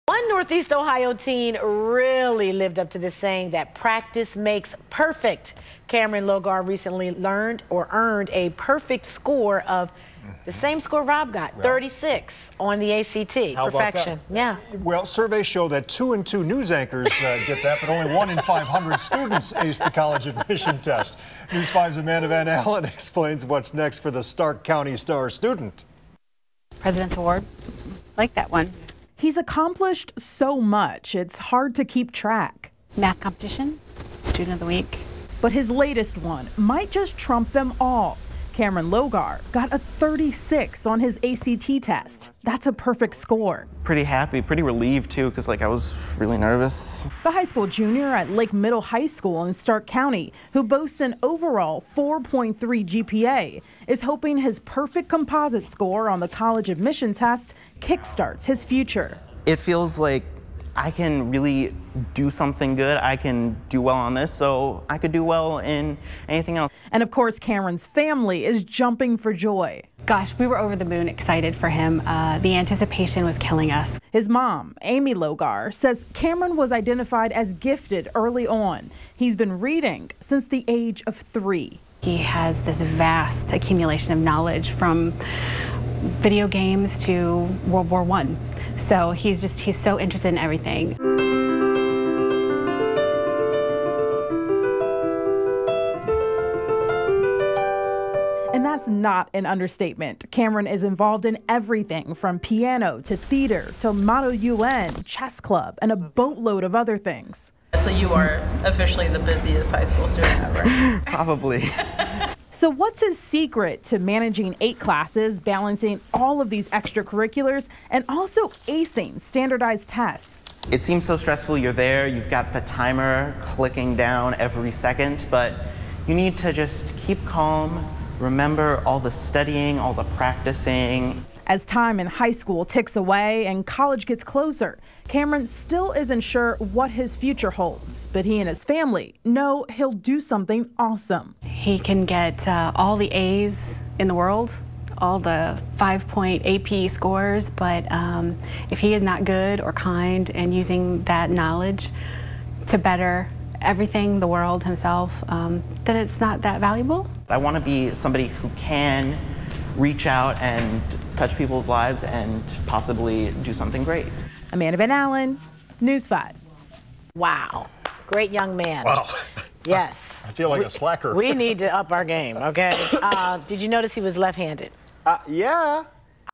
Stark County student aces act test
The high school junior at Lake Middle High School in Stark County, who boasts an overall 4.3 GPA, is hoping his perfect composite score on the college admissions test kick starts his future.